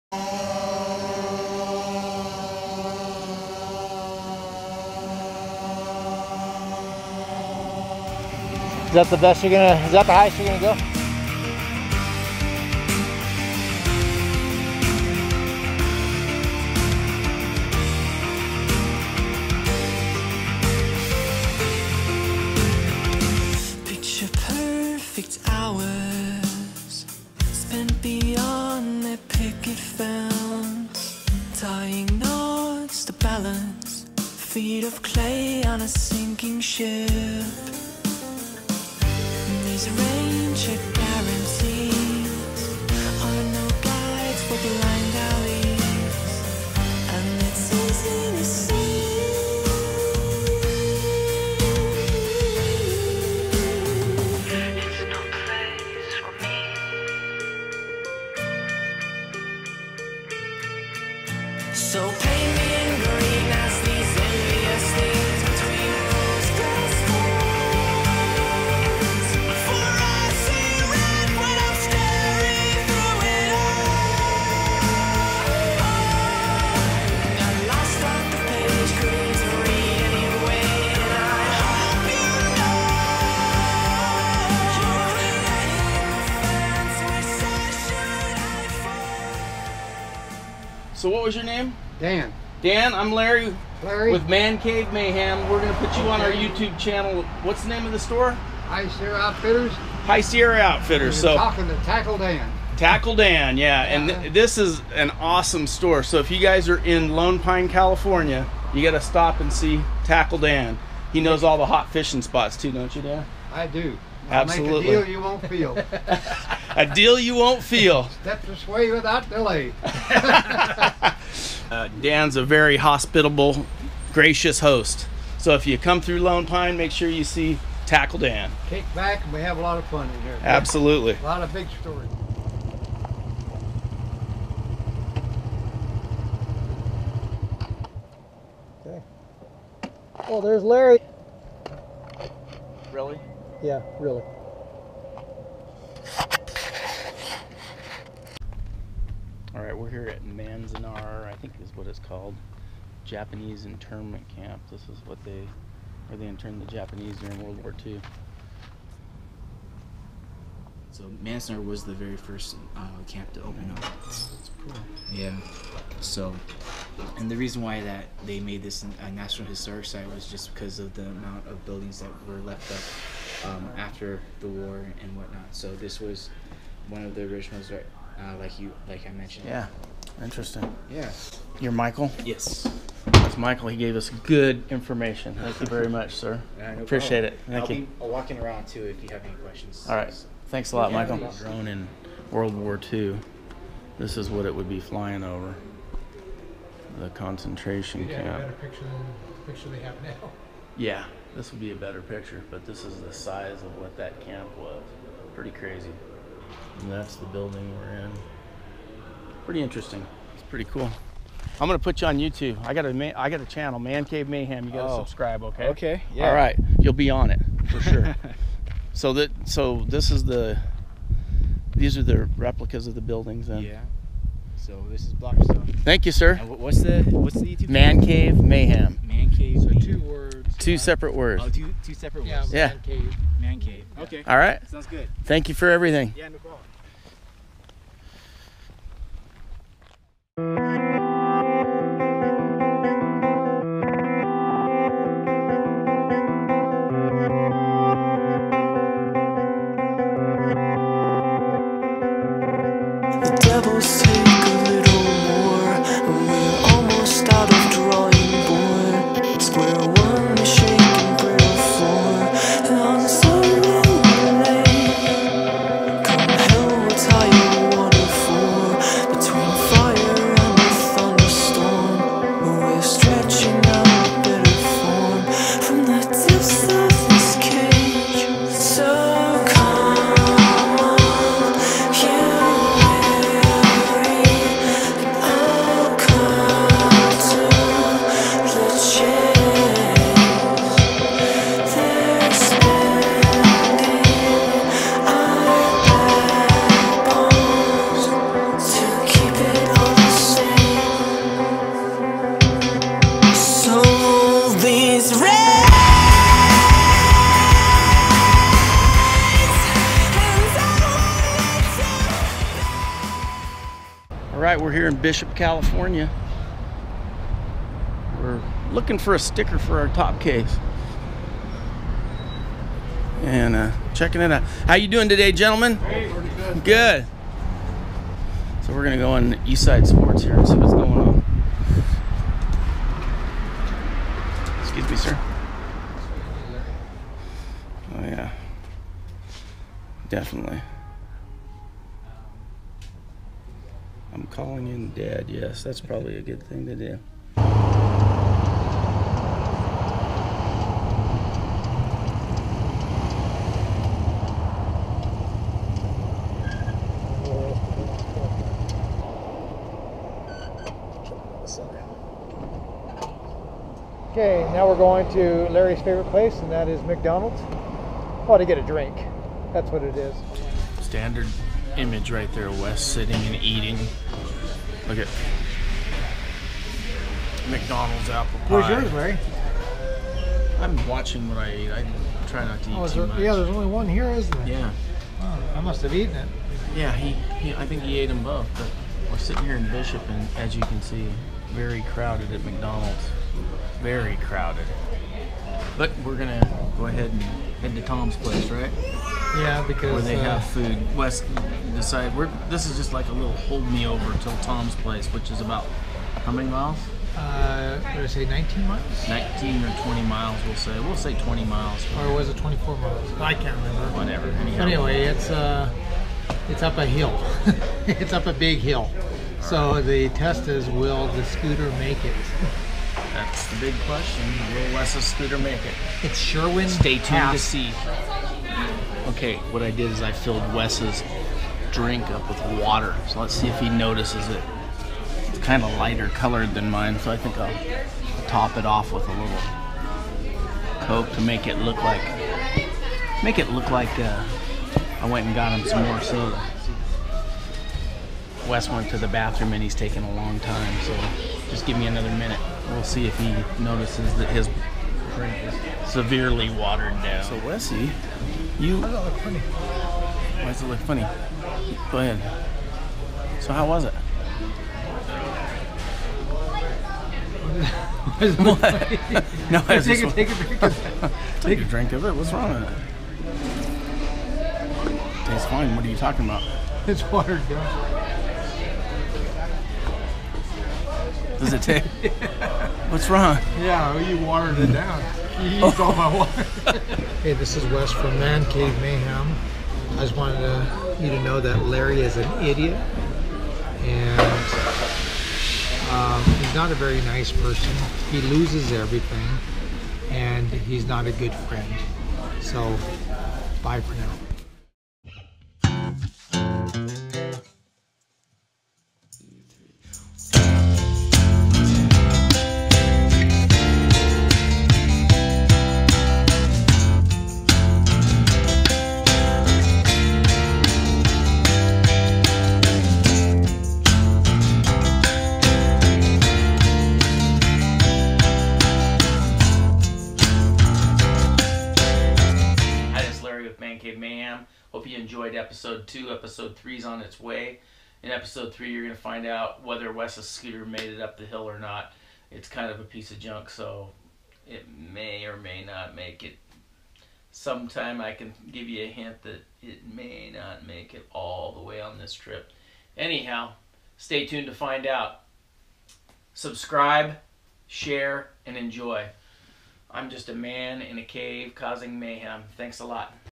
Scooter Camping Trip 700 miles to Mammoth and Back Episode #2
This is the second episode of our 700 mile scooter camping trip where we meet some great people and get some amazing drone footage. I hope you enjoy!